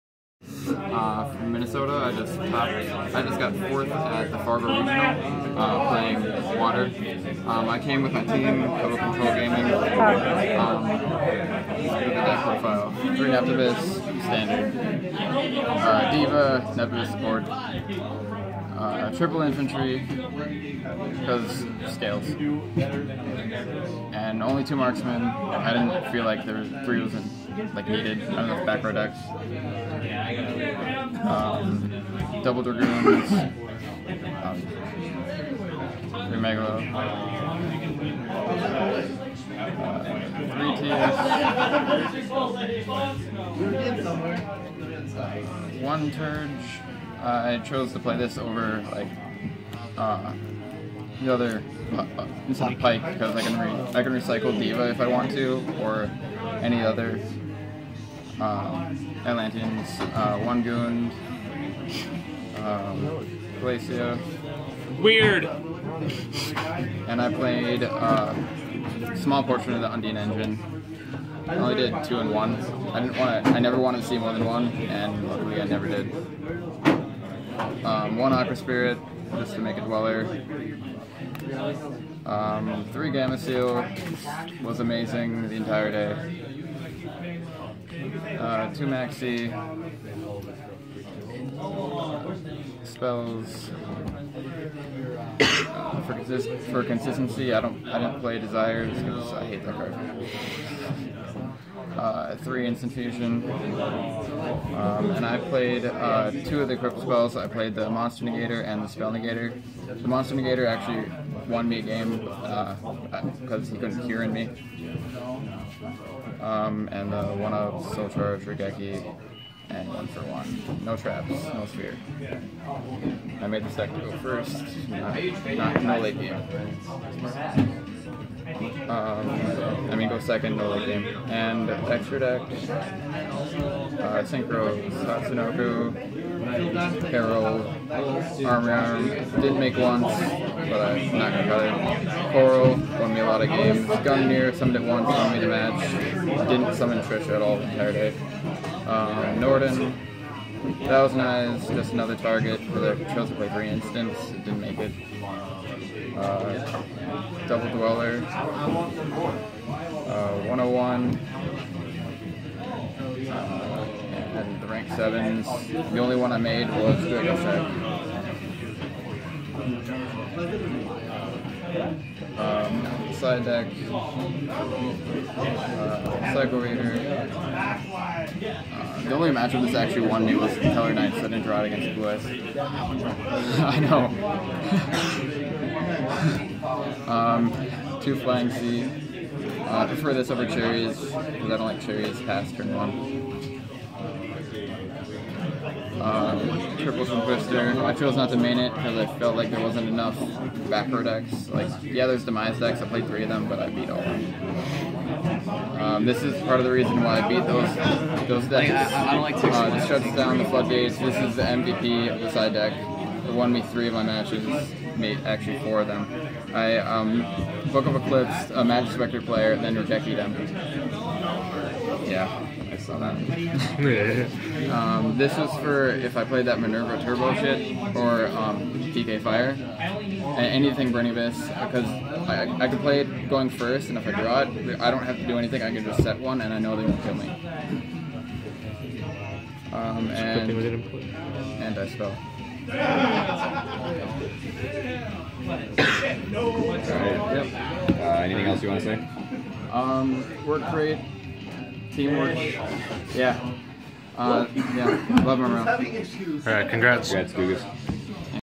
4th Place Fargo, ND Regional Deck Profile - Mermails - Josh Dugas
Congrats to Josh for snagging another top with his Mermail deck.